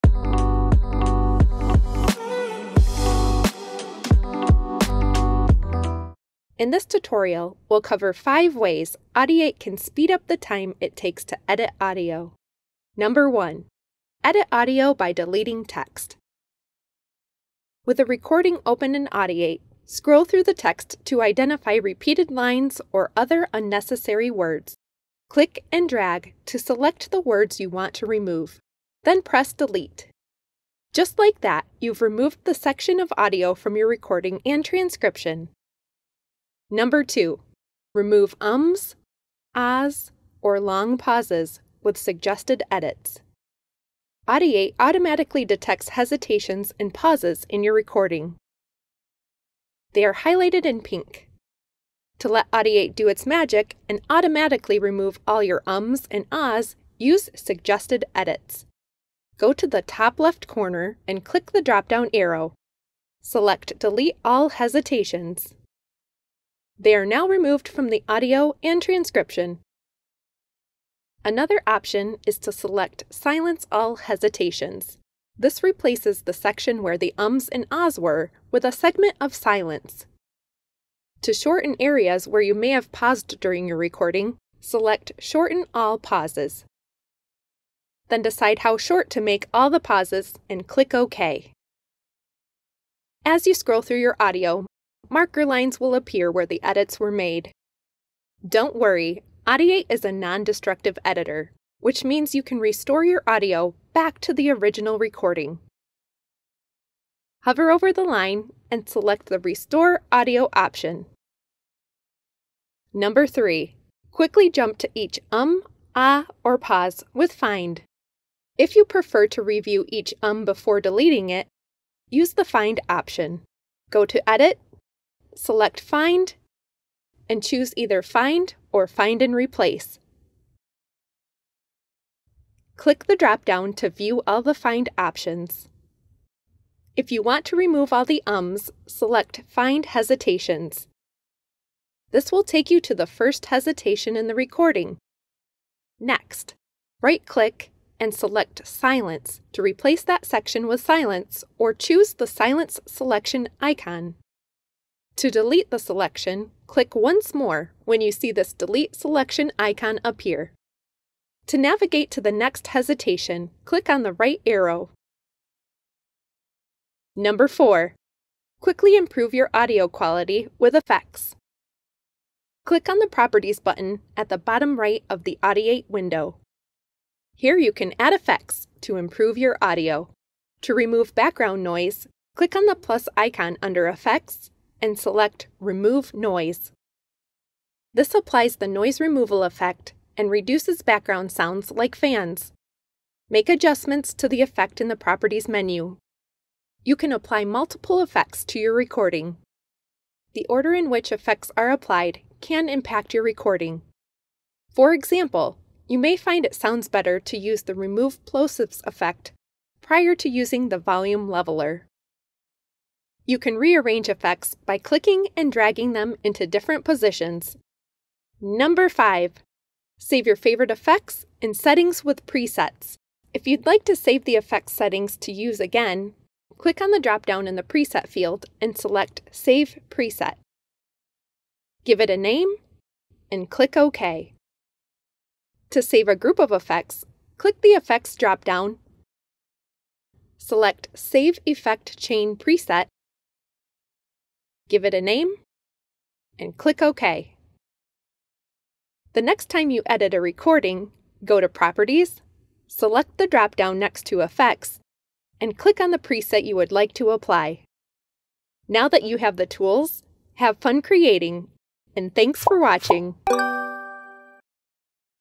Edit Audio Faster Than Ever [With Audiate]
In this video, Chandra from TechSmith shares 5 ways you can edit audio faster inside of Audiate than any other audio editor.

Whether it's how Audiate automatically transcribes your audio into text, its ability to then edit the audio like you're editing text in a document, or the automatic detection of "ums" and hesitations, Audiate will make your audio editing process way faster.

0:00 - Edit Faster with Audiate
0:16 - Edit audio like you'd edit text
0:39 - Audiate finds Um's and hesitations so you can delete them
1:57 - Automatically find mistakes
2:50 - Use built-in effects to improve your audio
3:41 - Save your custom effects as presets